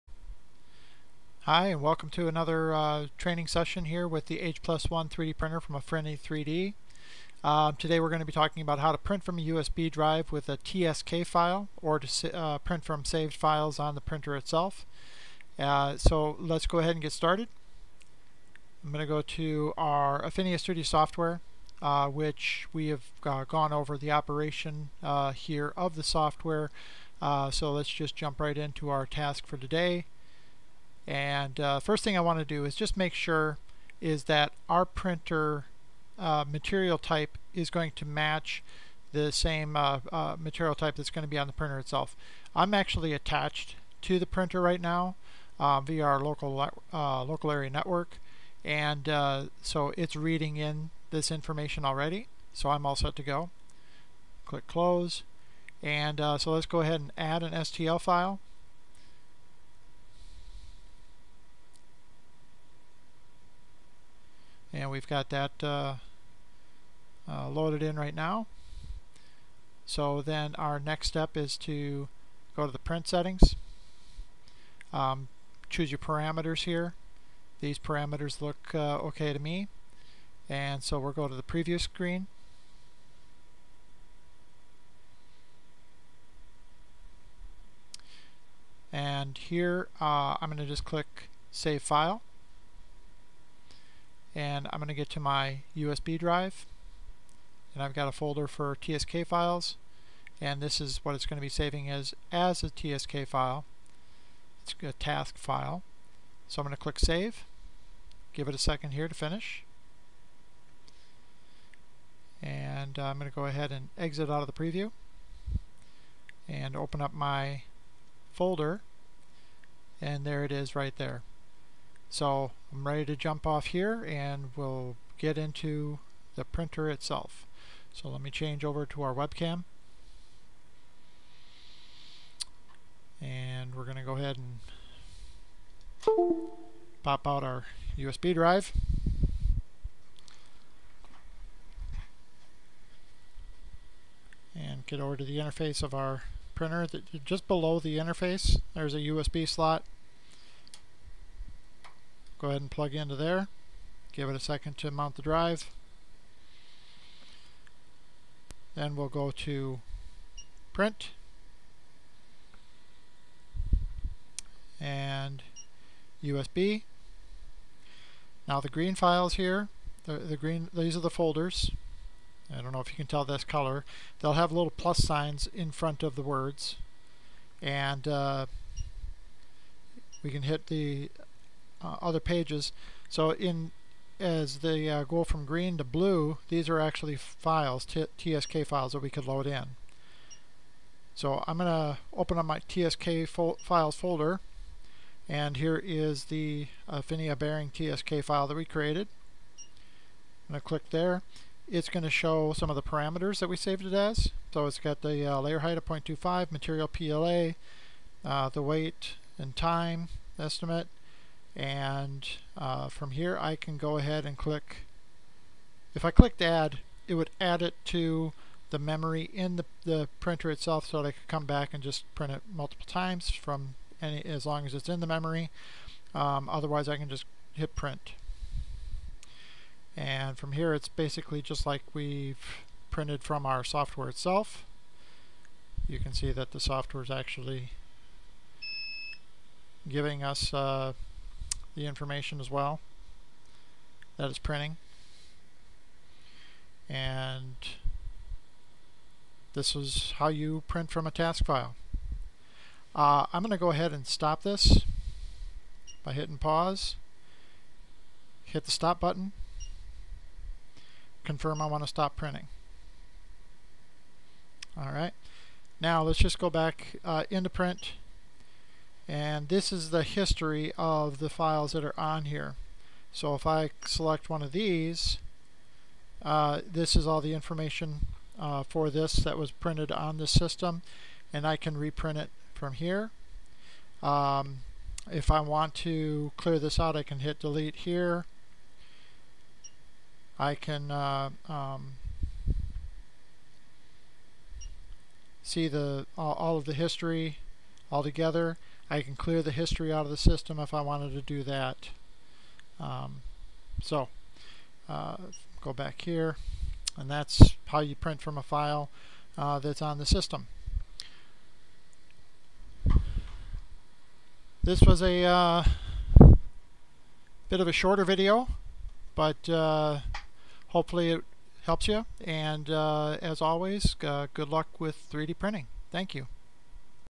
Afinia H+1 print from USB drive or printer history
This tutorial video details how to use Afinia Studio to create and save a TSK print file to a USB drive, then printing from the USB drive on an H+1 3D printer.  The video also shows how to print from the history stored on the H+1 3D printer.